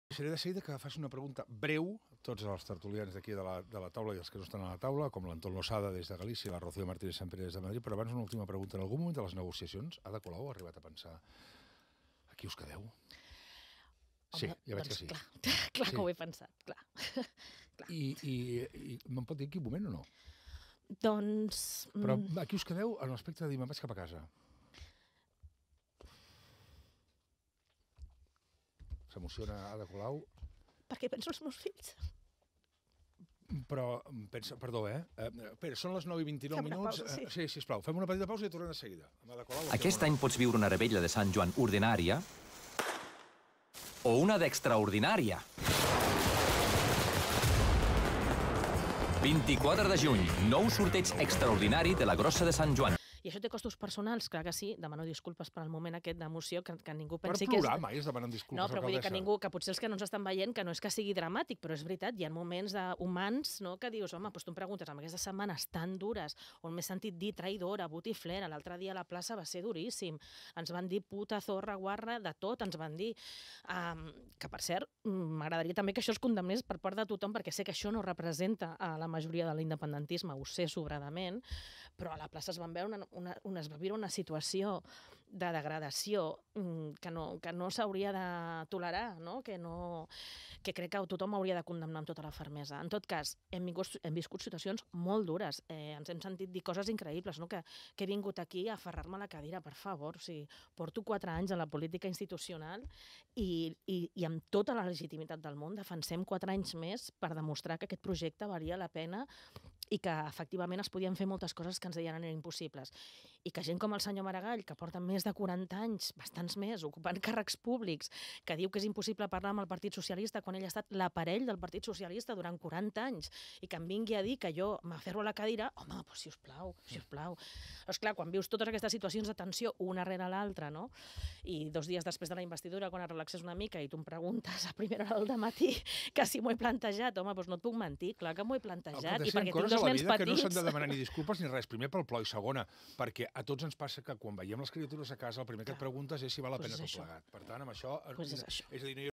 Colau se emociona y confiesa que pensó en dejarlo
La alcaldesa de Barcelona, Ada Colau, se ha emocionado durante una entrevista en Rac1 y ha confesado que en algún momento pensó en dejar la política.